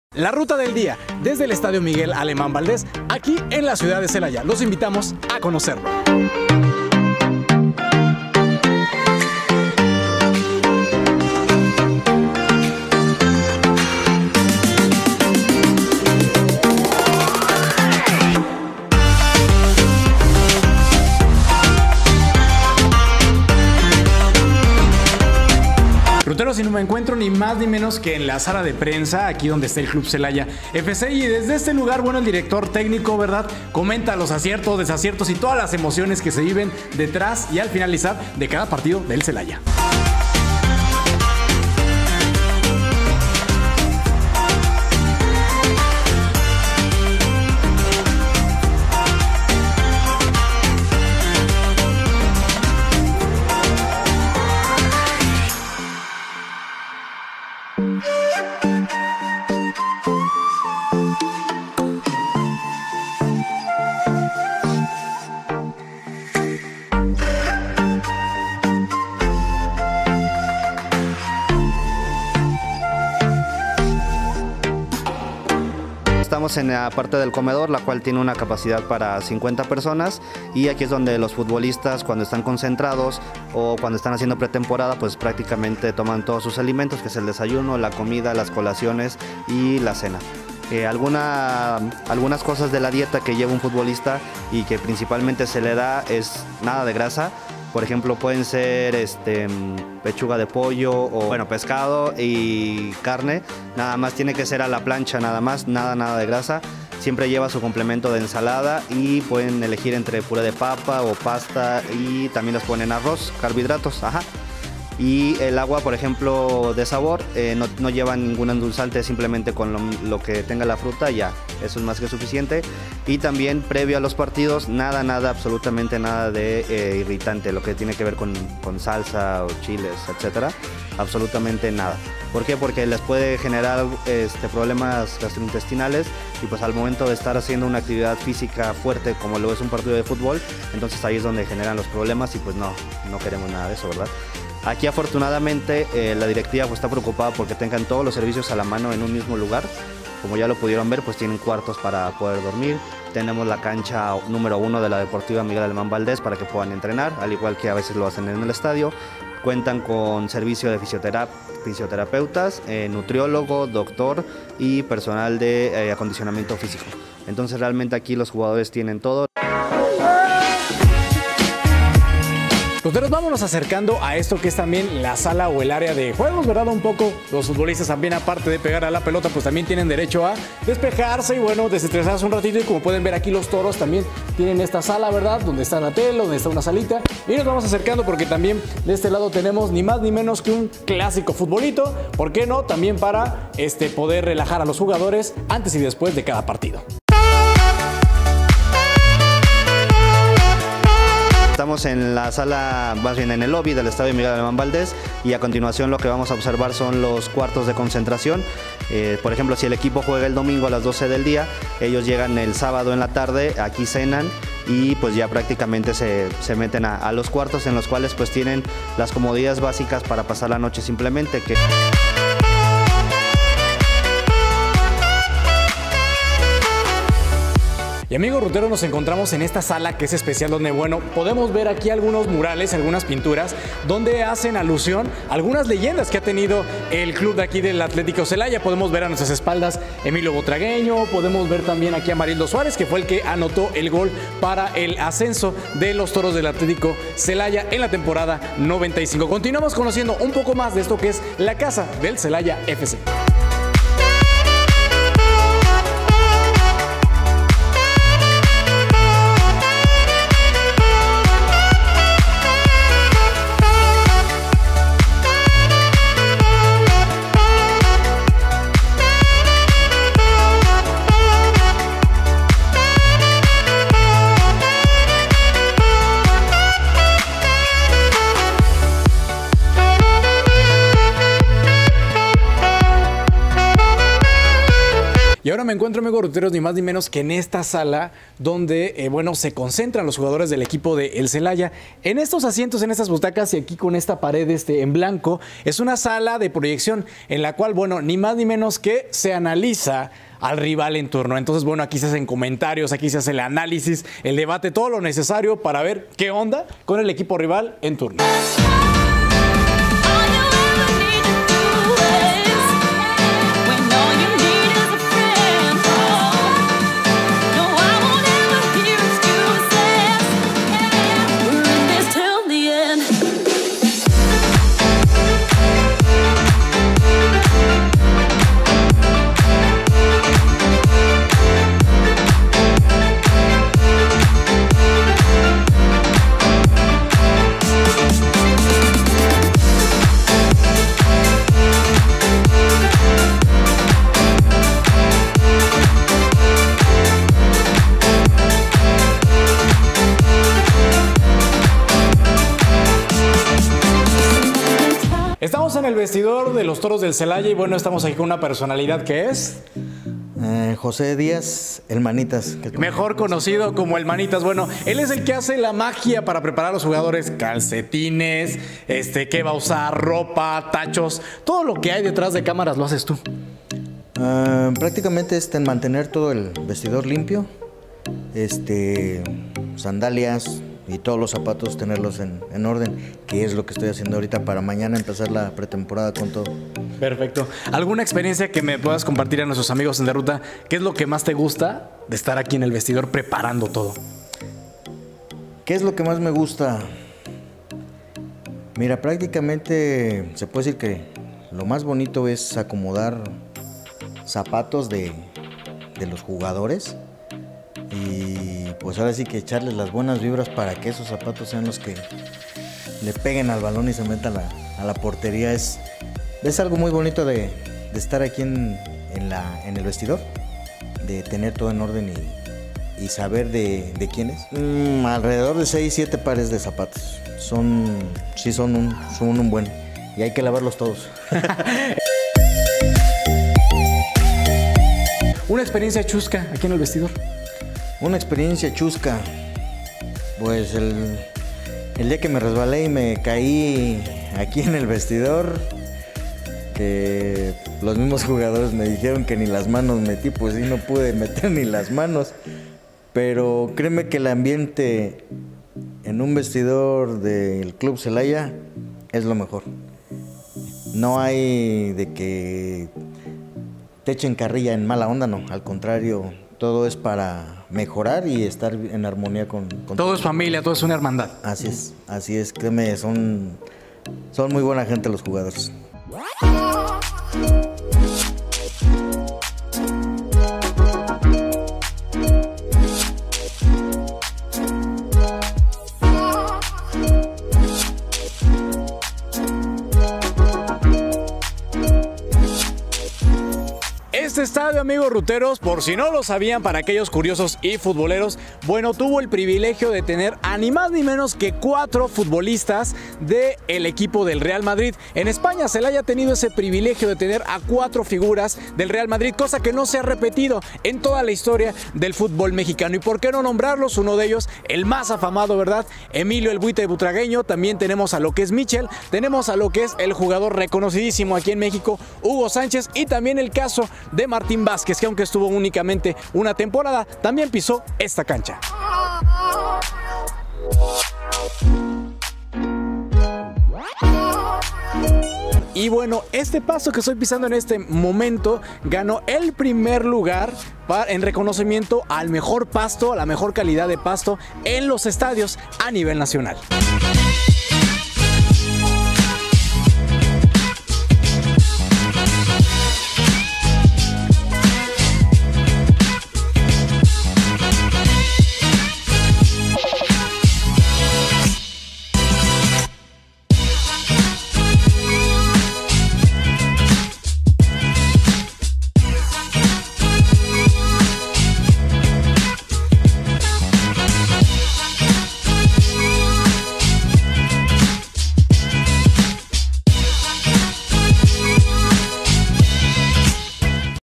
DE RUTA EN EL ESTADIO DE CELAYA
Un tour por el estadio MAV que fue pisado por grandes figuras del futbol internacional. Esta es la casa de los toros del Celaya ¡Acompáñanos!